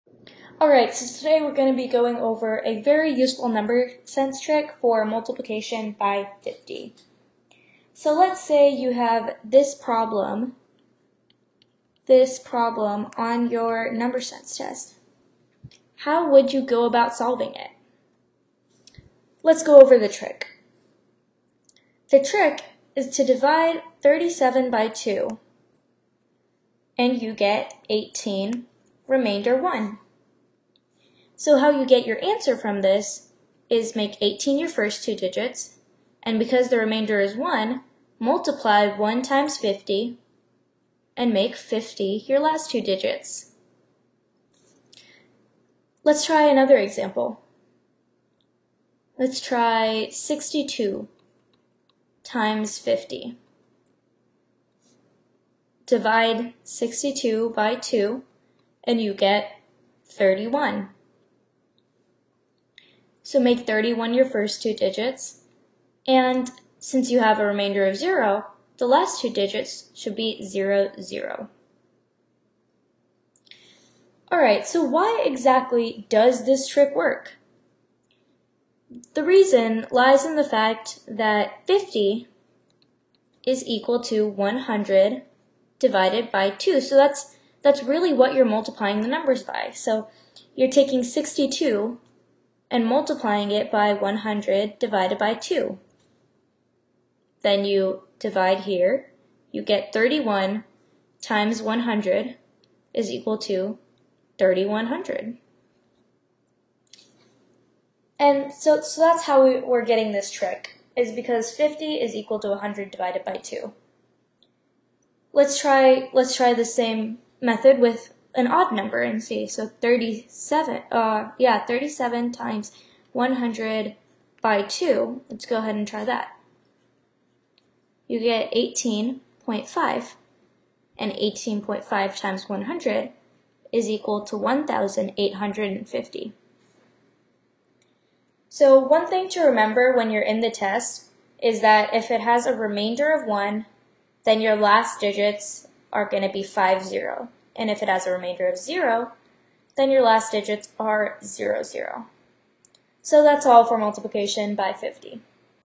COOL TRICK FOR UIL NUMBER SENSE: Multiplication by 50 real fast in your head
In this video, we will learn how to multiply numbers by 50 in our heads, quickly, with a number sense trick!

Join me in my learning journey as I learn -- share -- learn more.

Check out the Number Sense playlist for more tricks!

You can learn more about T2L Academy at: t2lacademy.org